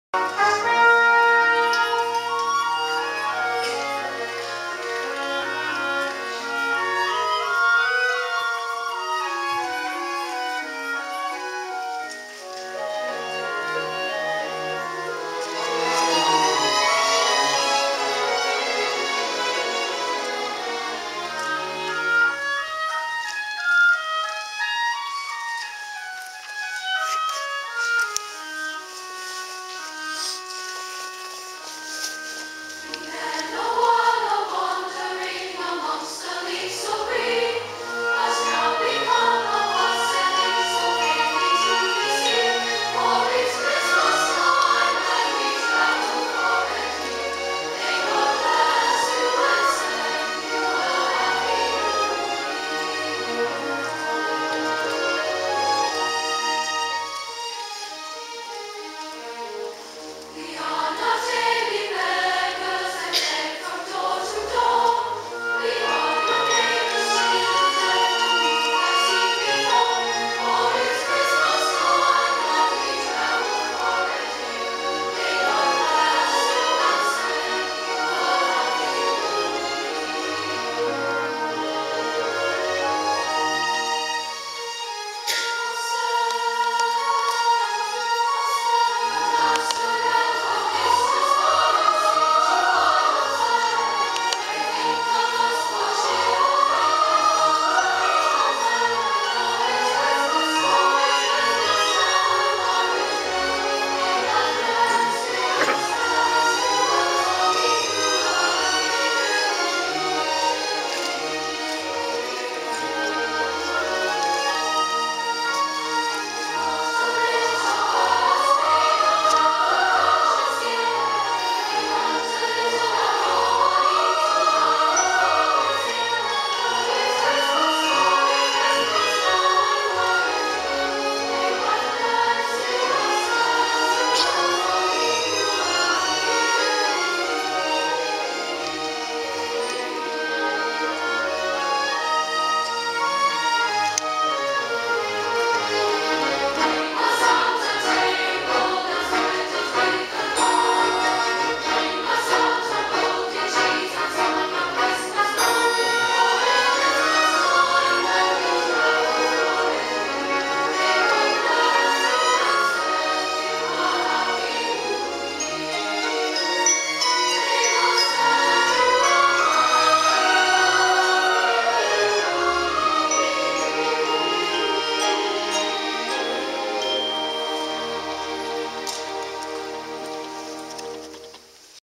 Children's Christmas Song
Northwestern Michigan Children's Choir with the Traverse Symphony Orchestra; December 2012